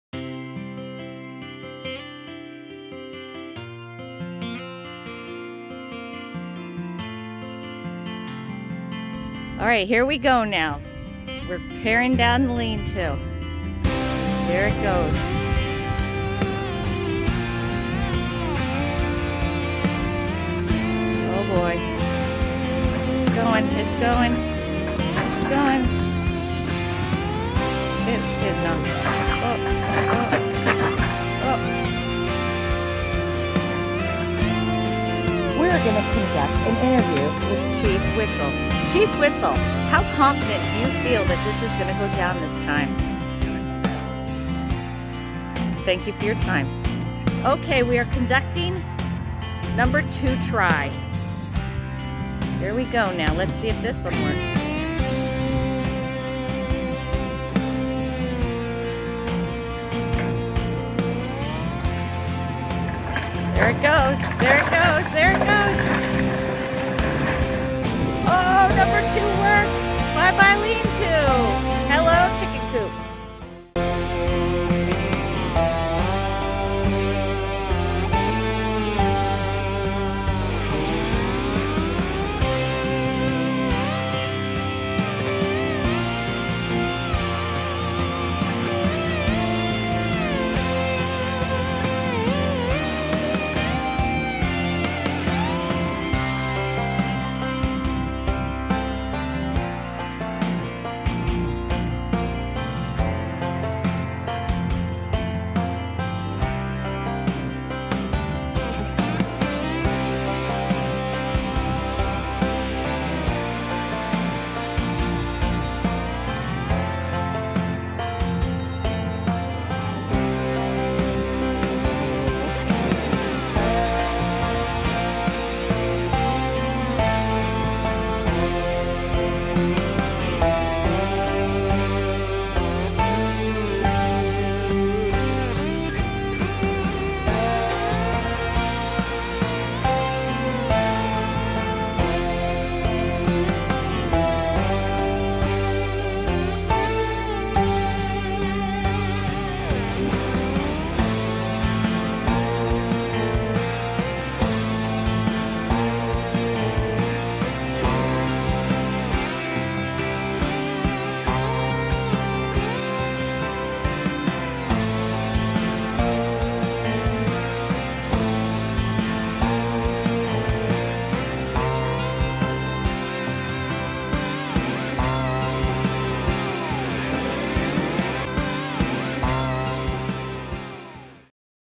May 28, 2023
Restoration of our 100 year old barn.